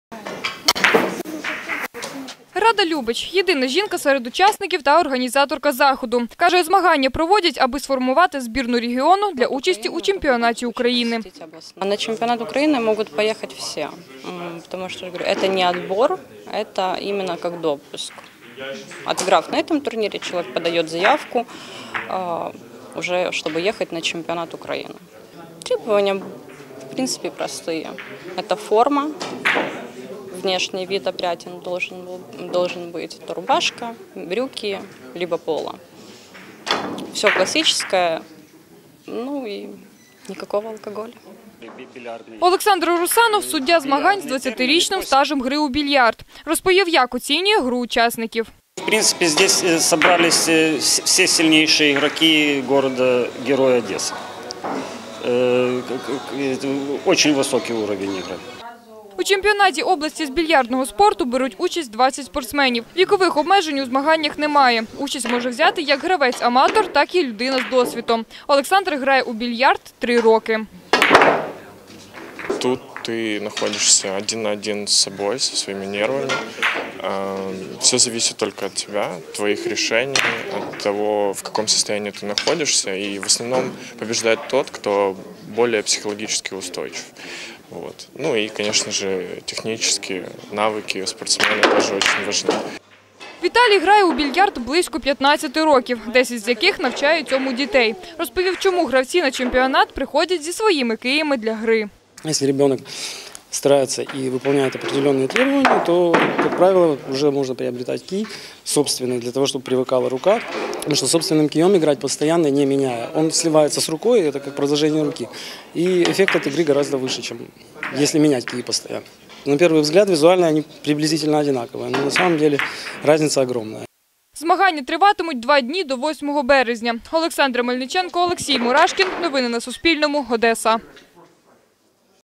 В Одесі проходить чемпіонат з більярдного спорту
Чемпіонат області з більярдного спорту серед чоловіків та жінок проходить в Одесі. У змаганнях беруть участь спортсмени з Одеси та Роздільної, передає кореспондентка Суспільного.

За словами єдиної жінки серед учасників та організаторки обласних змагань Ради Любич, змагання проводять аби сформувати збірну регіону для участі у чемпіонаті України.